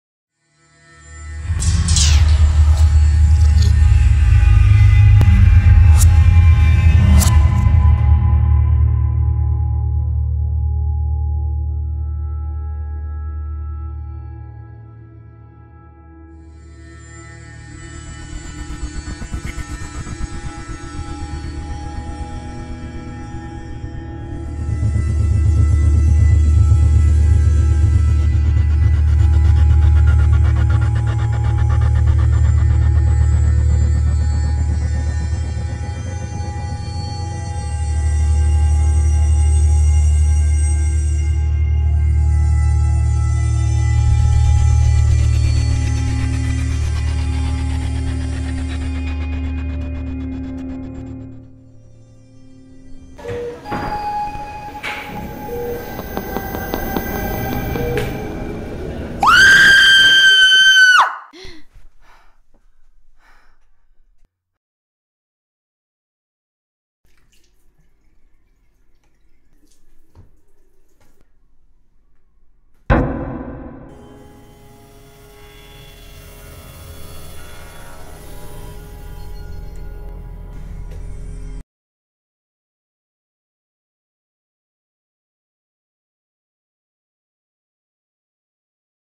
Corridors
The opening scene and title sequence from an original neo noir film created by Hannah Griffiths and Naomi Parker for their AS Media Studies production.
Written, Directed and Filmed by Hannah Griffiths and Naomi Parker
Edited by Hannah Griffiths
Starring Emily Tuff and Jenna Edwards